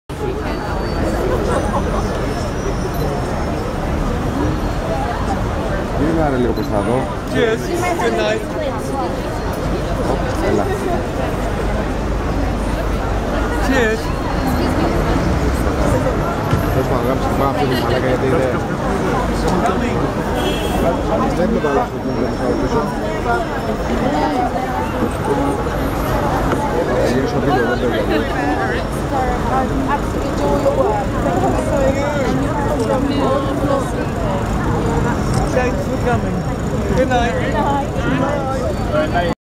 Sir Derek Jacobi Meets Fans Outside Garrick Theatre In London 4.06.2016
Sir Derek Jacobi Meets Fans Outside Theatre In London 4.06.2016